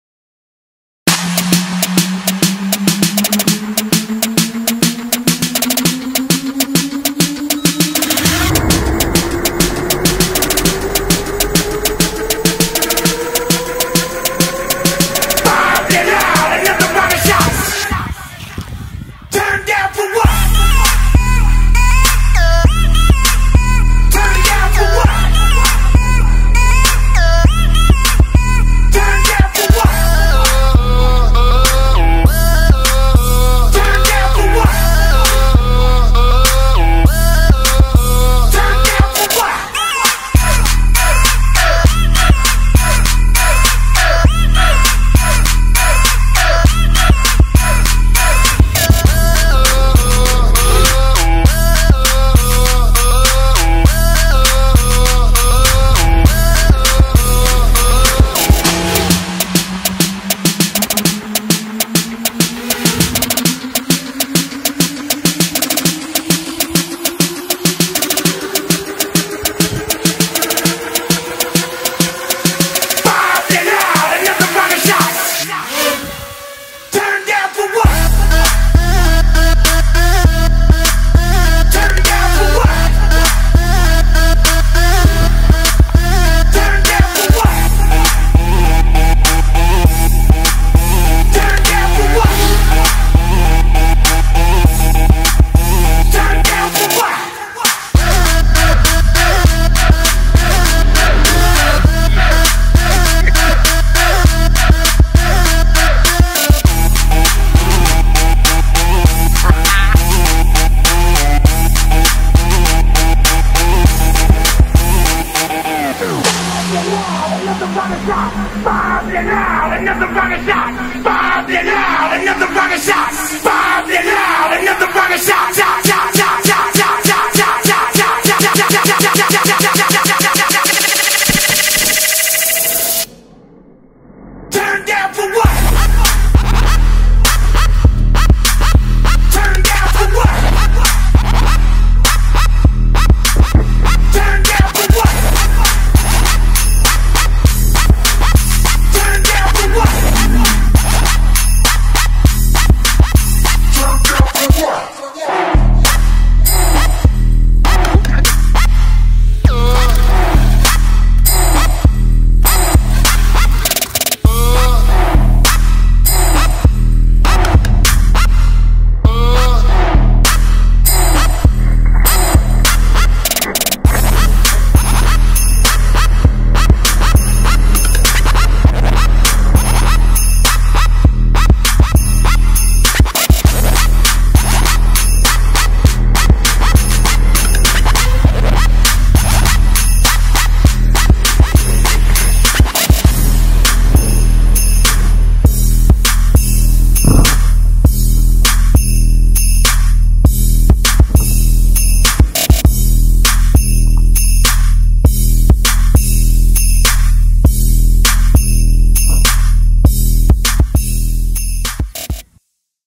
Paso por puertas!
Paso por puertas con drones de carreras, muy rapidos, muy duros y algunos aporrizajes 😂